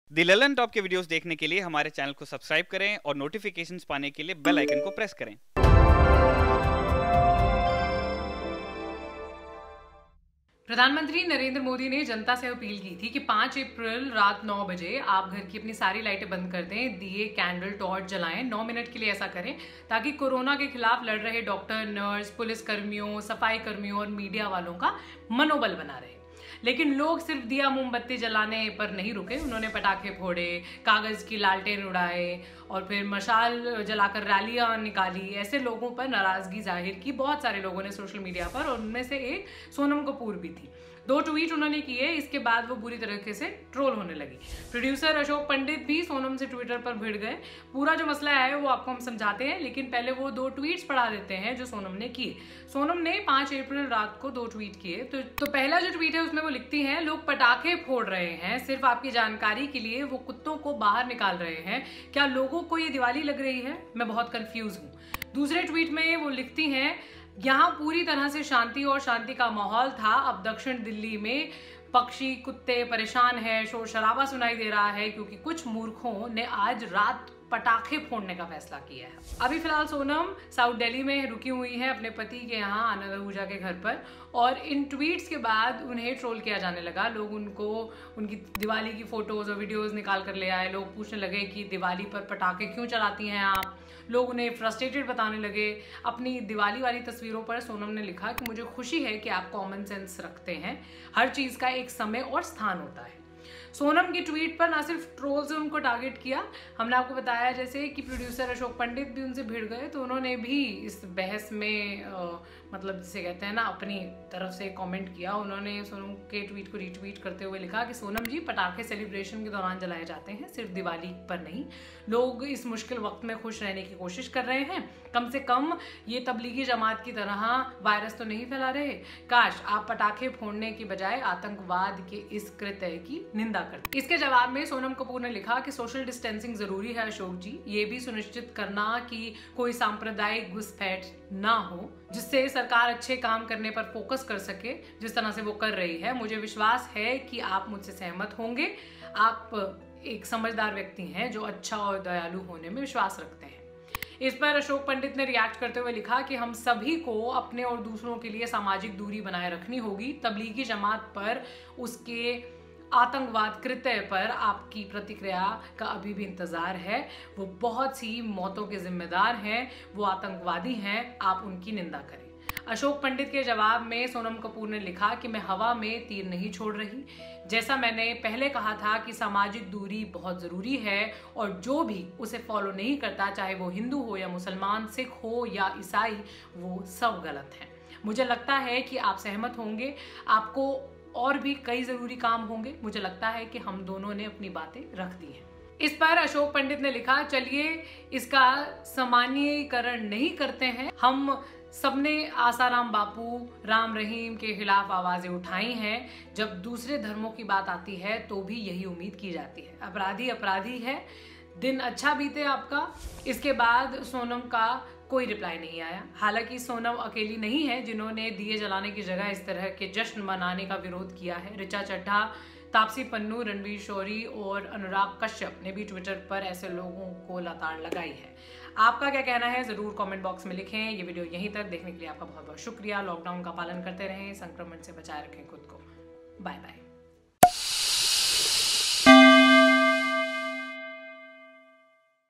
Sonam Kapoor से बहस पर 9pm 9minute activity में Ashoke Pandit Tabligi Jamaat घसीट लाए
Coronavirus in India: Bollywood actress Sonam Kapoor has been trolled for sharing her concern for dogs, birds and speaking against the people bursting crackers amid 9baje 9minute activity on April 5.

Sonam Kapoor expressed her anger by tweeting against cracker bursting during 9pm9minutes activity by tweeting. The producer Ashoke Pandit started an argument on Twitter to which Sonam Kapoor gave a befitting reply. Watch the video for more details.